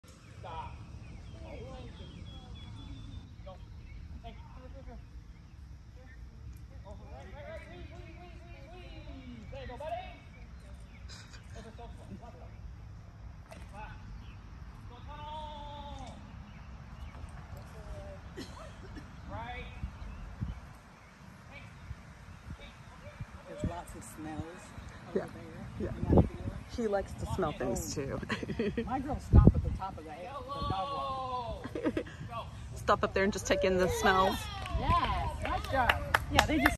Nitro, NADAC Agility; Novice Regular Round 2 Q
Nitro, German Shorthaired Pointer (GSP)

10 June  2022

Novice Proficient (20”) Regular

Judge: Becki Uberger

Location: Canines By Karen, Greenback, TN

Q-10, Time: 36.23 seconds, YPS: 2.90

2nd place